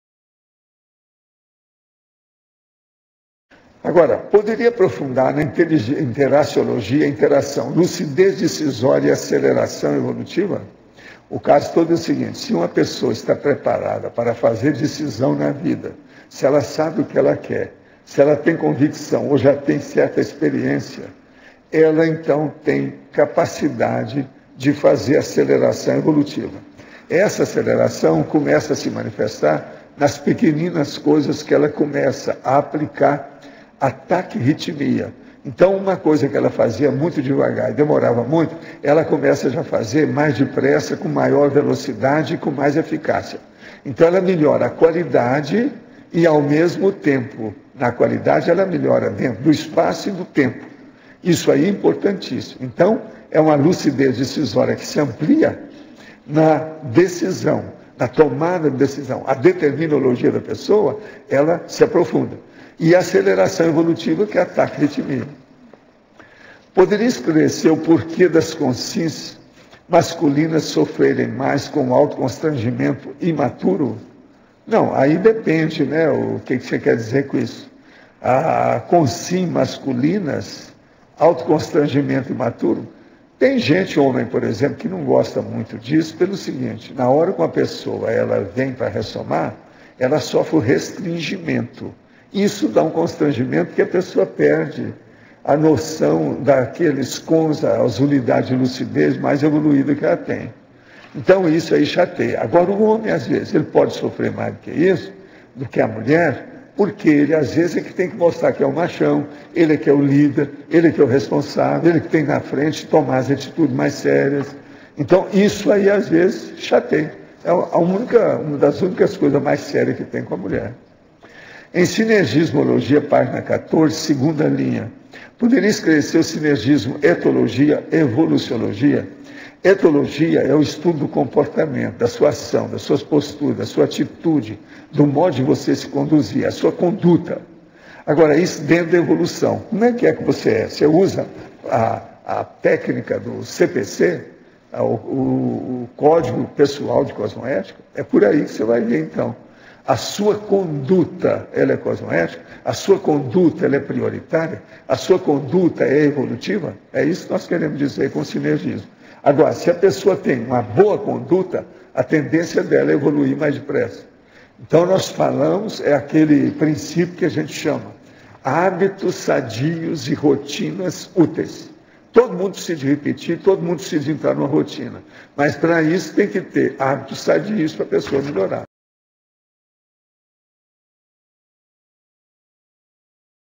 CONSCIENCIOLOGIA - Aceleração EVOLUTIVA - TAQUIRRITIMIA - Etologia - EVOLUCIOLOGIA
Minhas perguntas: Poderia ESCLARECER o porquê das conscins masculinas sofrerem mais com o autoconstrangimento imaturo? Em sinergismologia página 14 segunda linha, poderia ESCLARECER o sinergismo etologia-EVOLUCIOLOGIA?

Tertúlia - Aula 2102 - Terça-feira - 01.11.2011 - Duplicidade de objetos (Autexperimentologia) - Ao vivo todos os dias de forma gratuita nesse horário, 12:30 às 14:30...Clique lá e fique mais sábio(a)...Tertúlias com o prof. Waldo Vieira